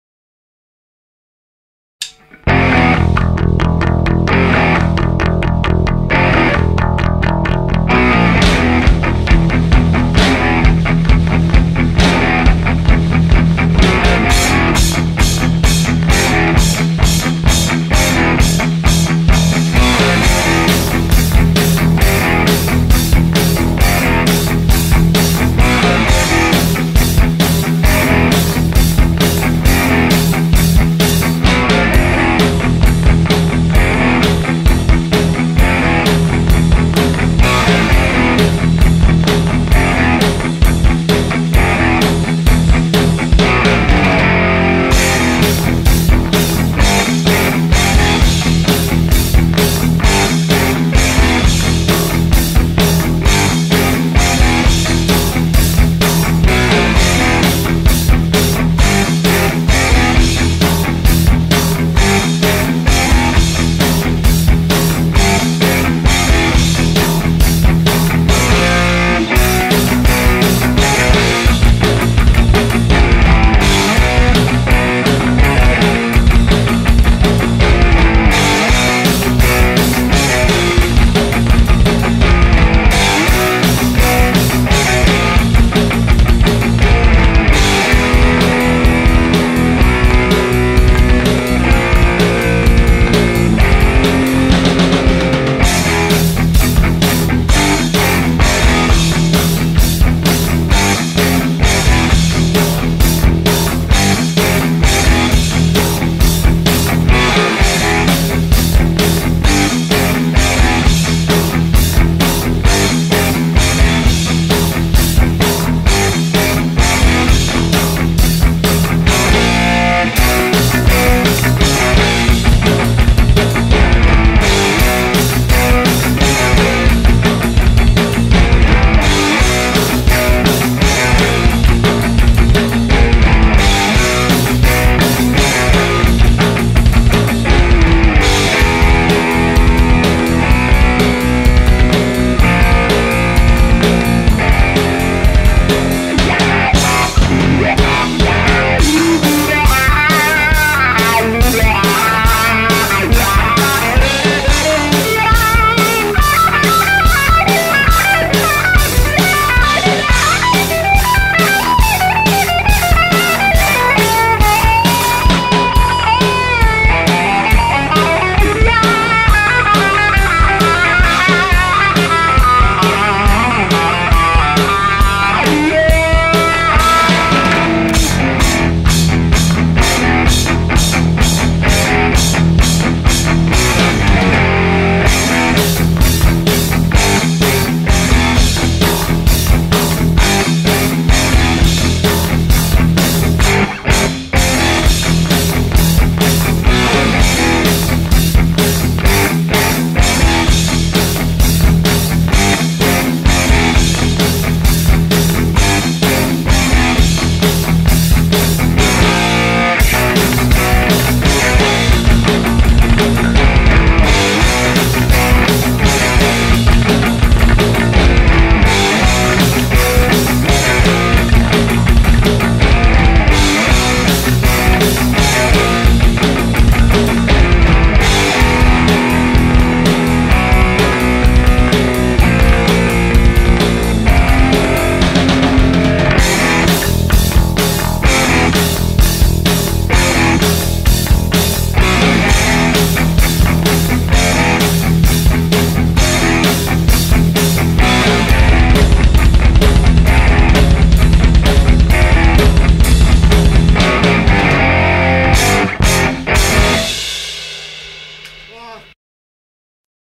Megashit - demo
Bass - Andras Bognar
Guitar - Oravecz Richard
Drums - David Vigh
We are looking for a singer!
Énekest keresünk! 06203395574